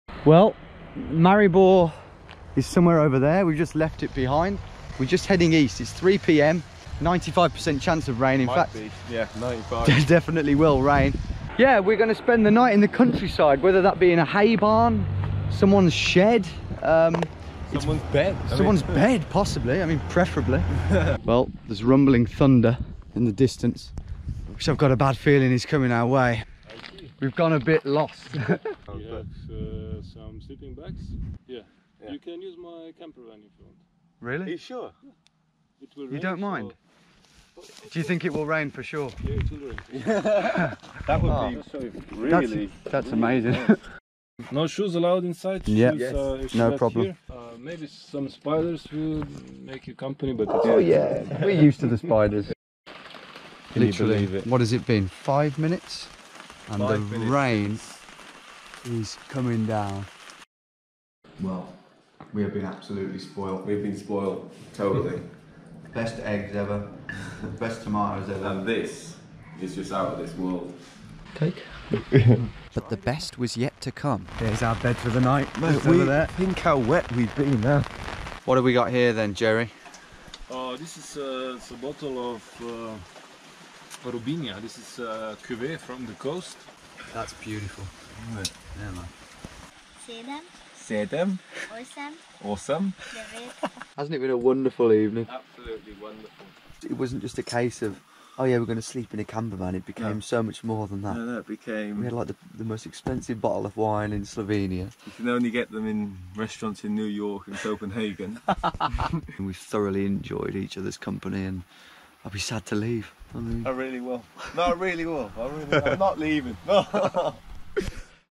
"You can sleep in my campervan" (Jerry from Slovenia restores our faith in humanity)
Taken from How not to travel Europe #4